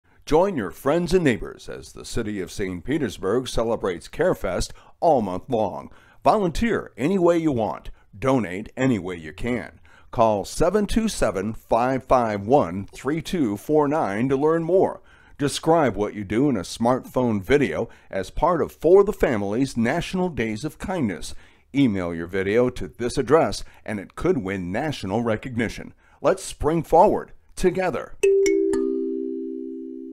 2021 CareFest Spring PSA
Carefest Spring is only one way that St. Petersburg enables residents to help each other now that it means even more.  It continues throughout the month of March, and is just another reason why the city has earned international distinction as a City of Compassion.  This video by For The Family outlines how people can donate and volunteer to make St. Petersburg better during CareFest, and submit their video descriptions as part of Kindness of the Heart and the National Days of Kindness.  The NDK is a seasonal holiday that encourages people and organizations to summarize their kind and caring actions as examples that inspire us all.  They'll also be eligible to earn a Family Flame Award at the Sixth Annual National FAM Awards in November.  For The Family's partnership with St. Petersburg is joined by school systems, nonprofits, universities, faith-based organizations, businesses, government agencies, and other municipalities.  However, our biggest asset is people who know the value of showing their Kindness of the Heart.  People like you.